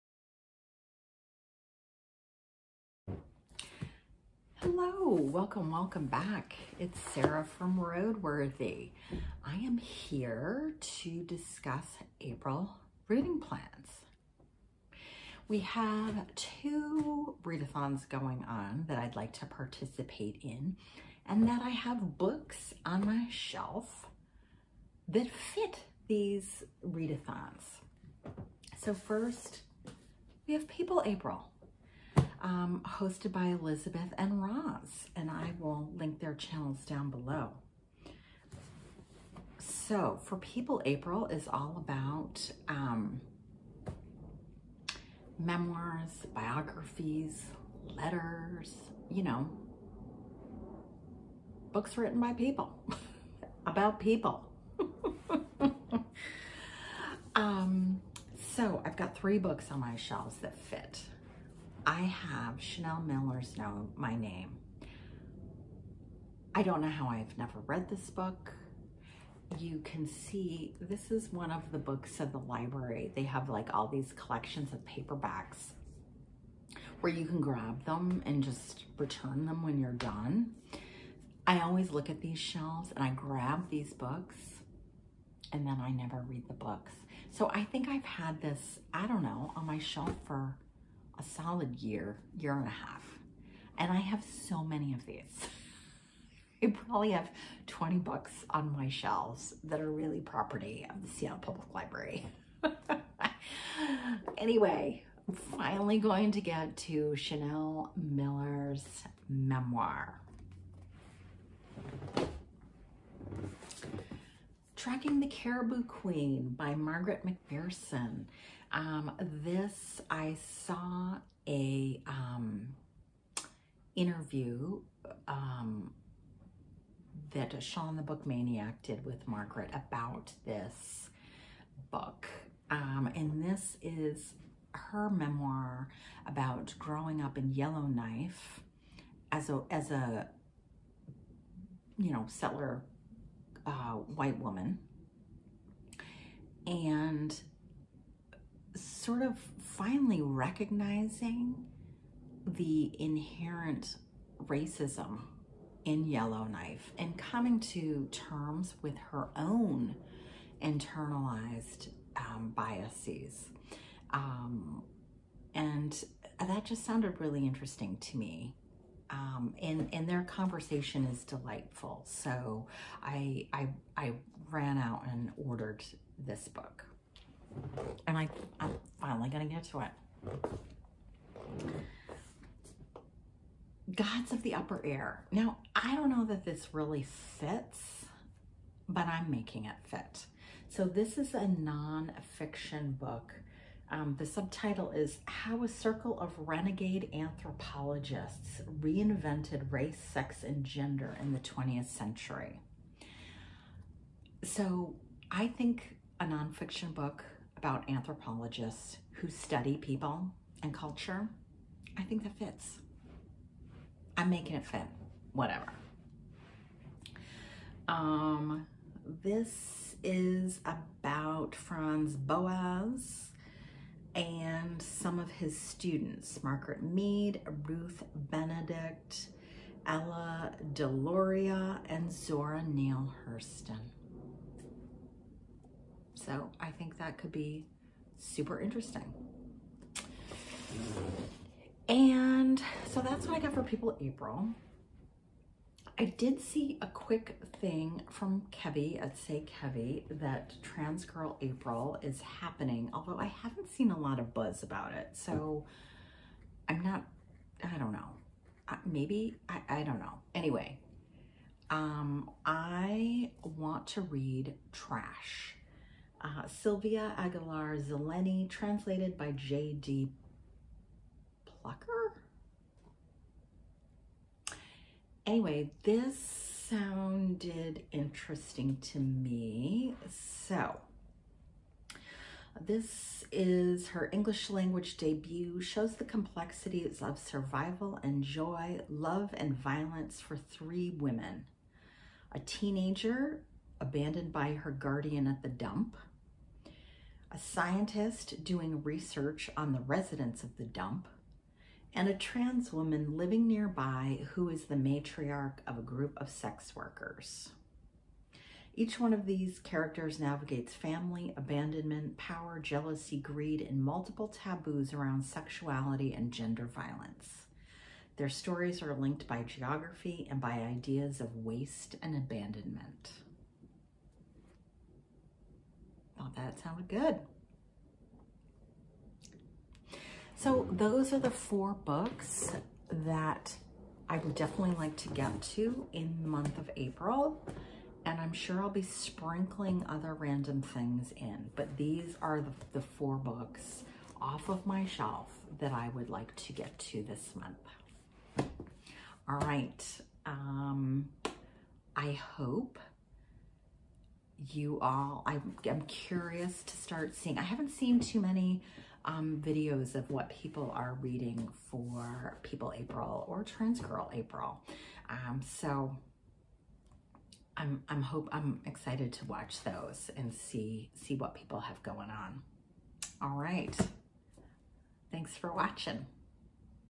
April TBR
Welcome as I discuss the books off my shelf that I hope to read for People April and Transgirl April.